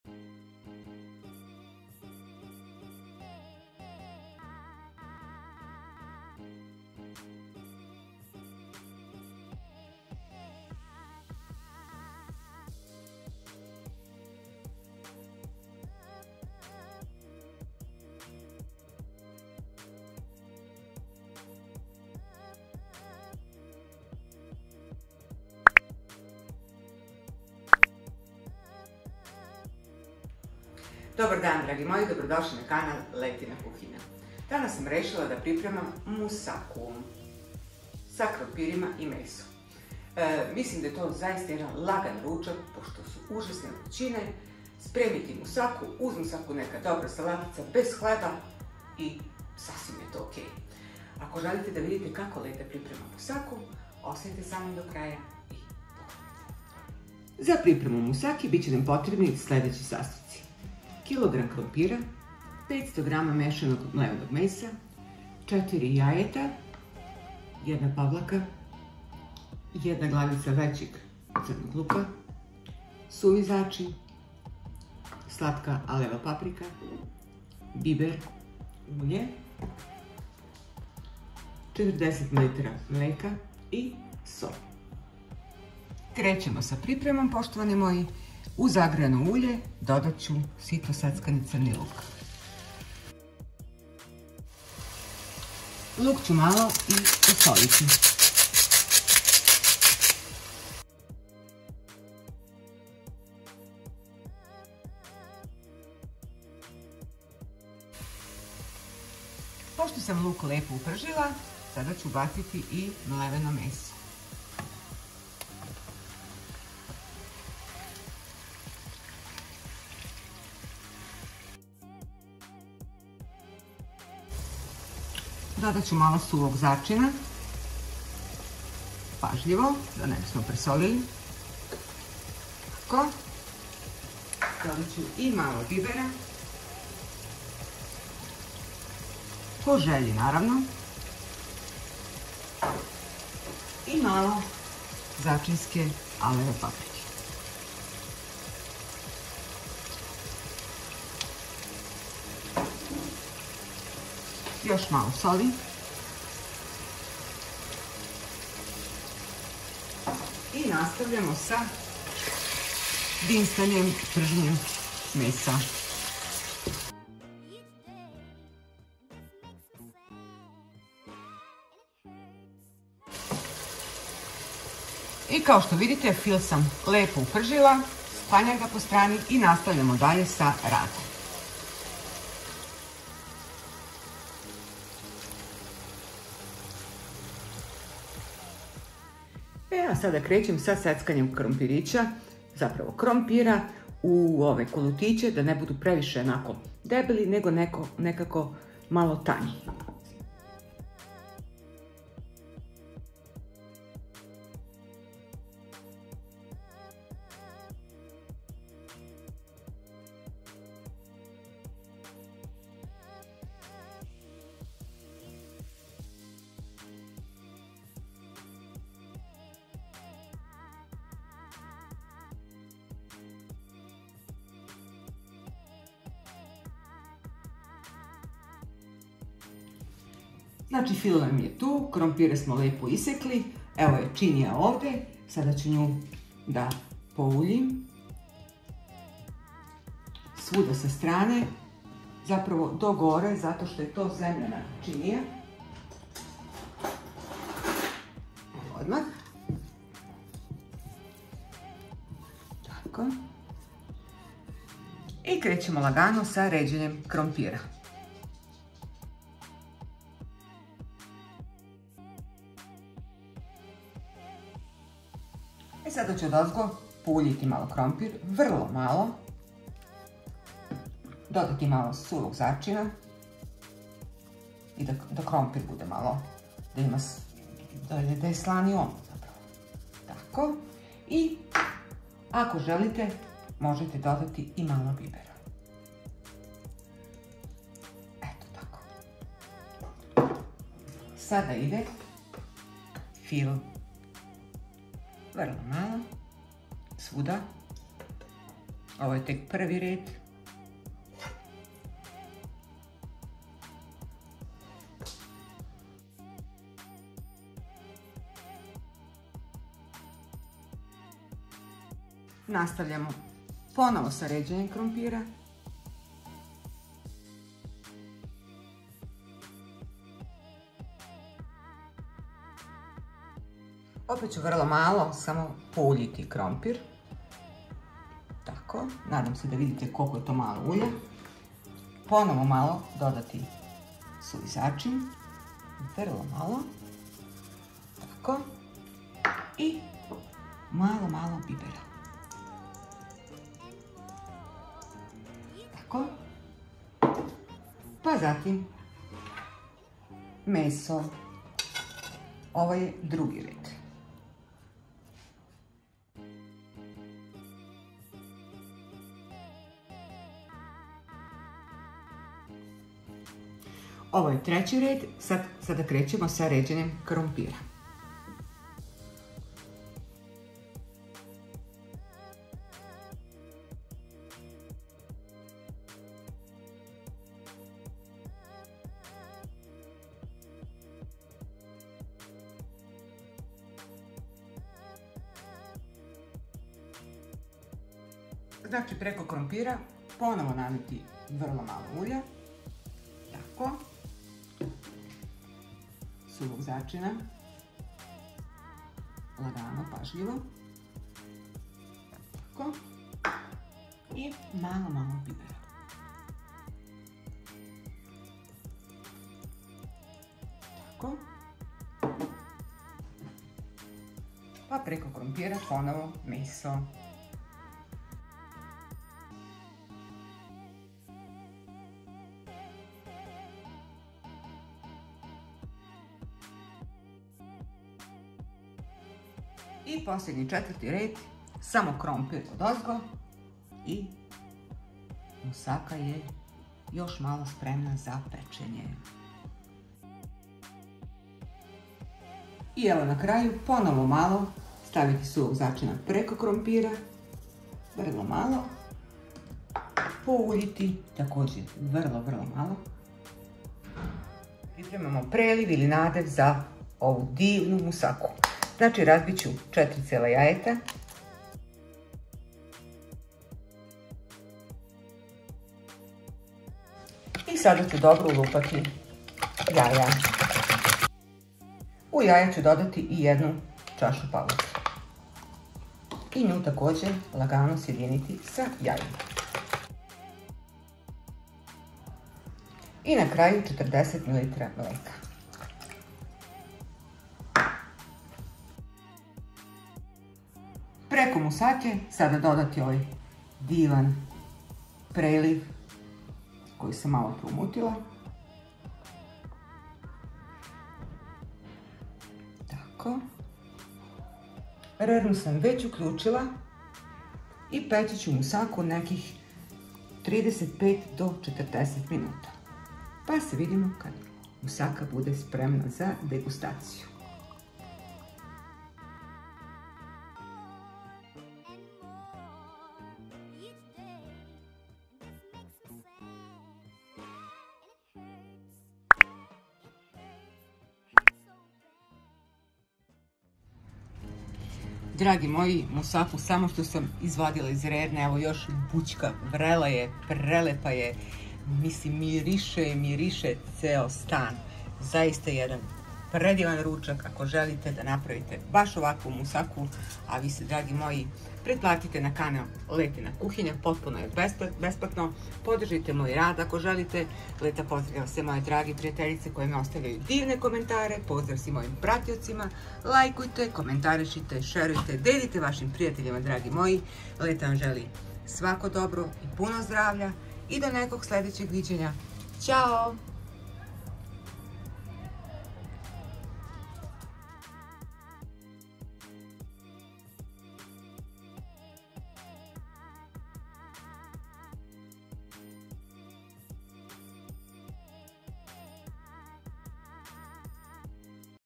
Musaka - RECEPT
Sastojci:
* 500gr mlevenog mesa
* 1kg krompira
* 1 glavica crnog luka
* 1 kisela pavlaka
* 4 jaja
* 40ml mleka
* so
* biber
* suvi začin
* slatka začinska paprika

Na ulju propržiti luk i meso, začiniti po ukusu. Krompire iseći na tanke kolutiće. Redjati po nauljenoj posudi prvo krompir, začiniti po ukusu, pa dodati meso i tako sve dok imamo materijala. Poslednji red su nam krompiri. U posebnu posudu umutiti jaja, dodati pavlaku i mleko i sve sjediniti. Preliti preko musake. Peći na 200 stepeni 45 minuta. Prijatno!